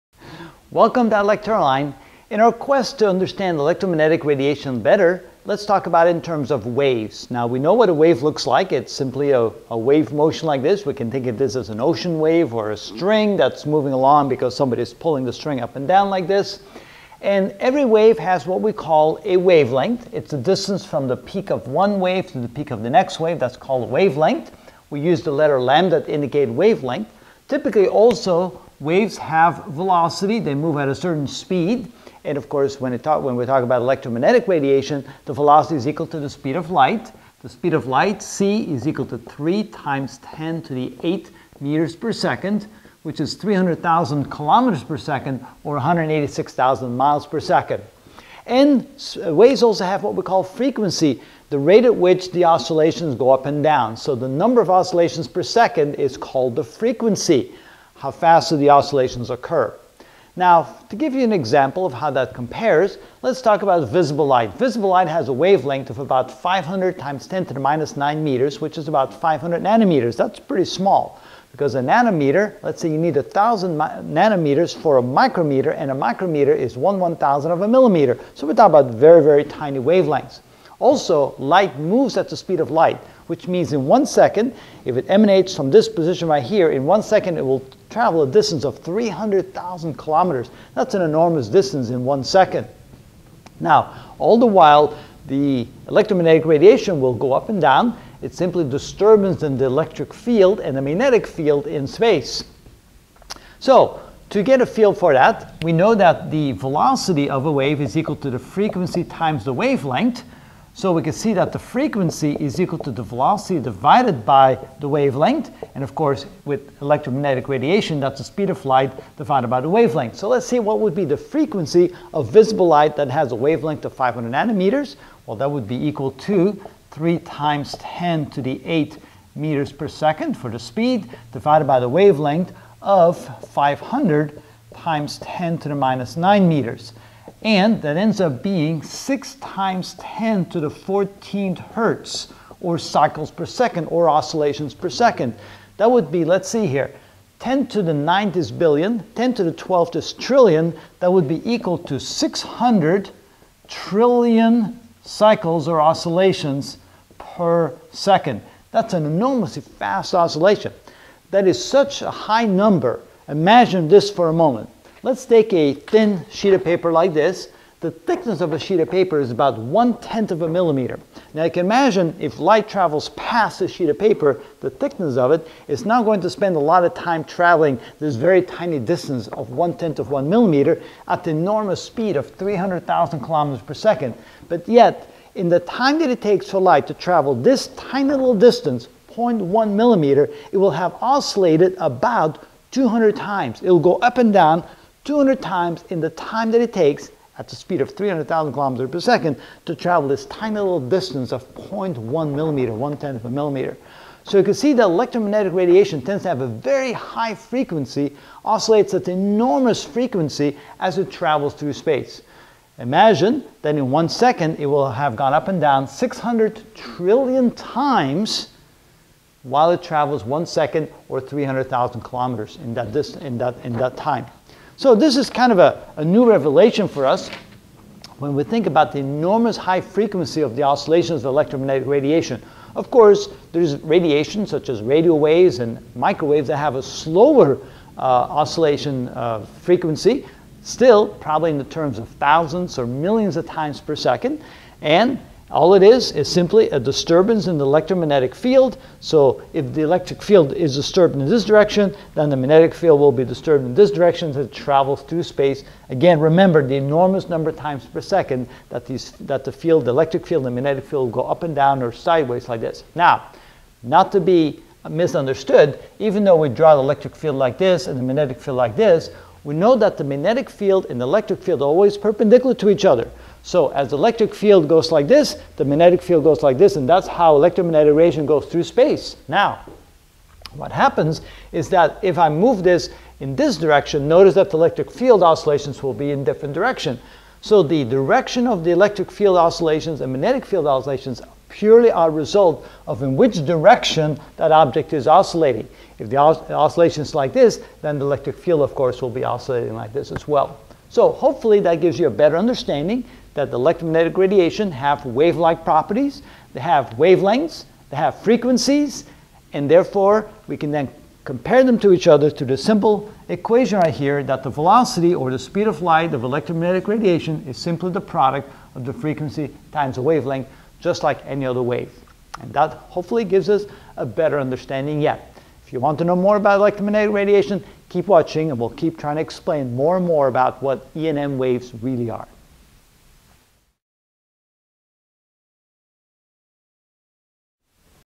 Physics 50 E&M Radiation (2 of 33) Frequency and Wavelength
In this video I will explain electromagnetic radiation in terms of frequency and wavelength.